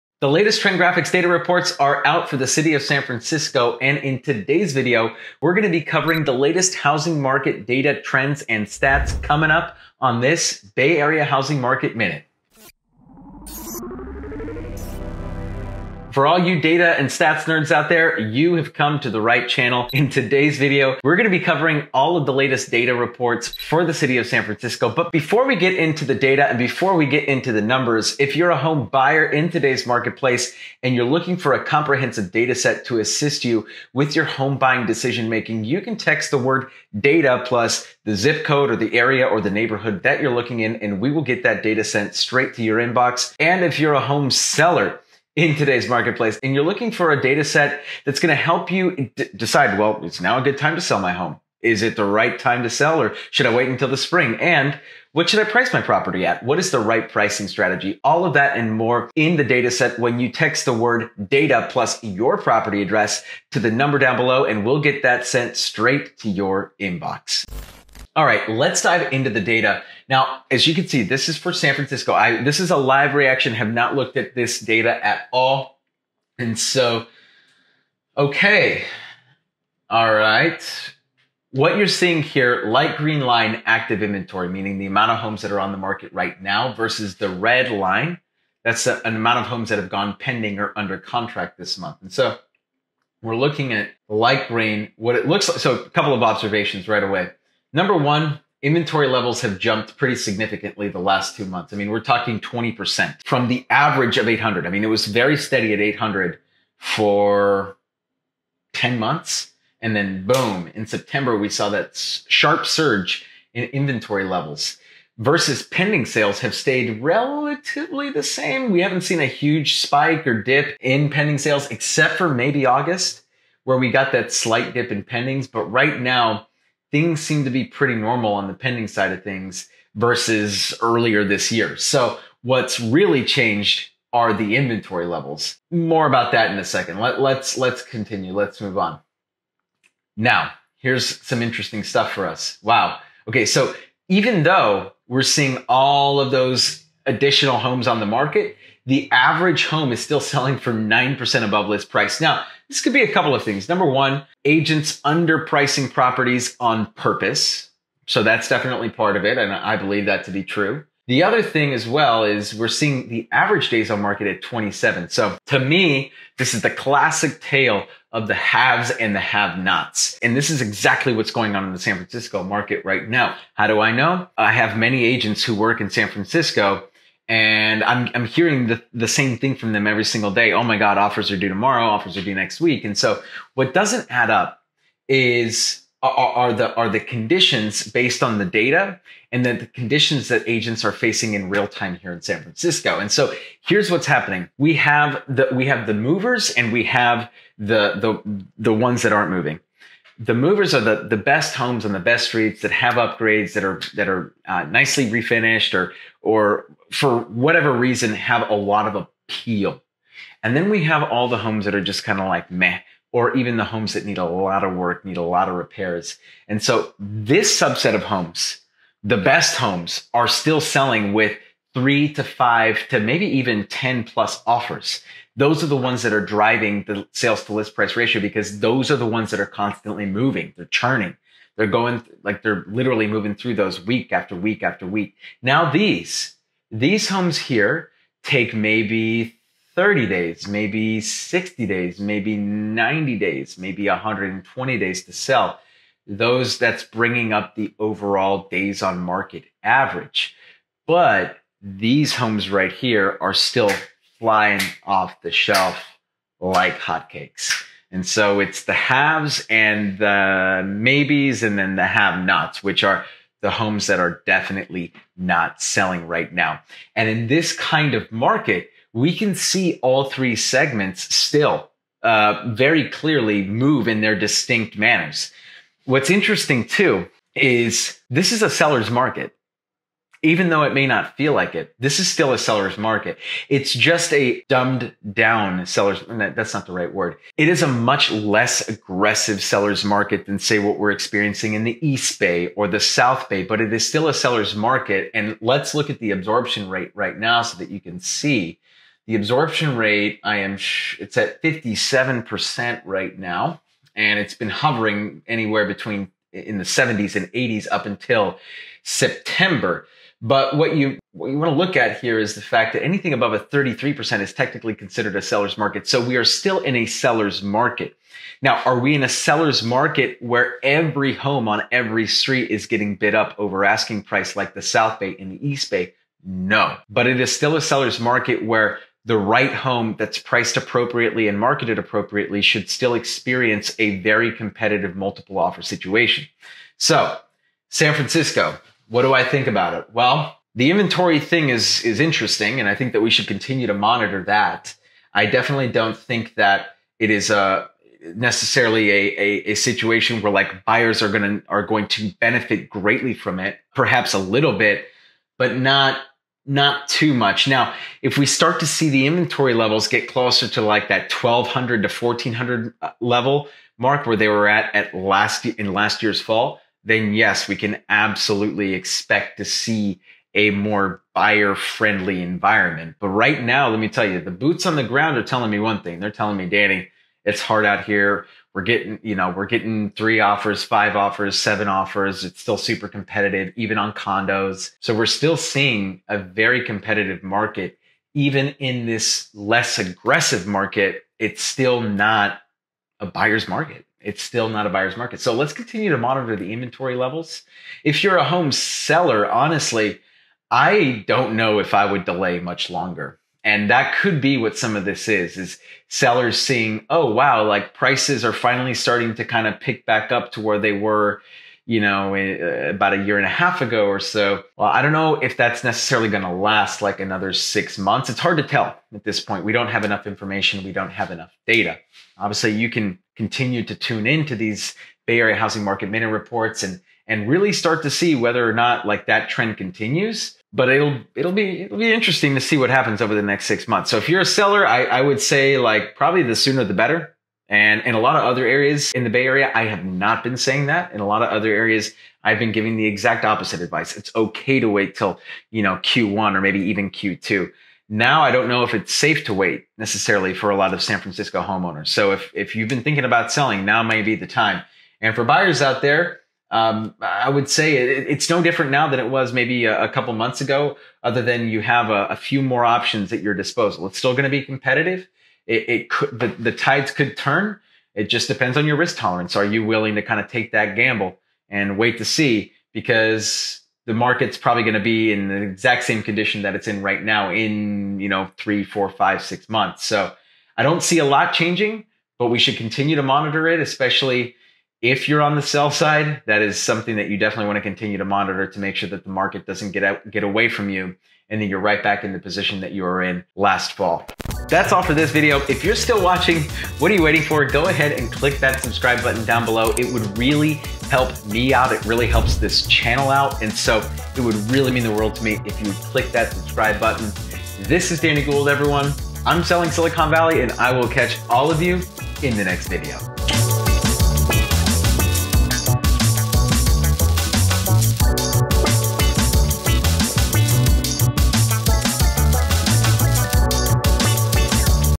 What Are The Conditions of THIS Seller's Market? | Bay Area Housing Report 2021
What are the conditions of the San Francisco housing market?! In this Bay Area Housing Market Minute, we're going over and analyzing the most recent data and numbers from the San Francisco market. Are you looking to buy or sell a house in the area? Find out whether NOW is the right time to do so! We're telling you when to buy, when to sell, and what to expect in the upcoming new year.

🔹Want MORE Data?🔹
Are you a Homebuyer?

Are you a Home seller?

🔸Need Advice? Contact Danny 🤓🔸

Snapchat 👉 TheGouldTeam

We’re SellingSiliconValley, one dream at a time!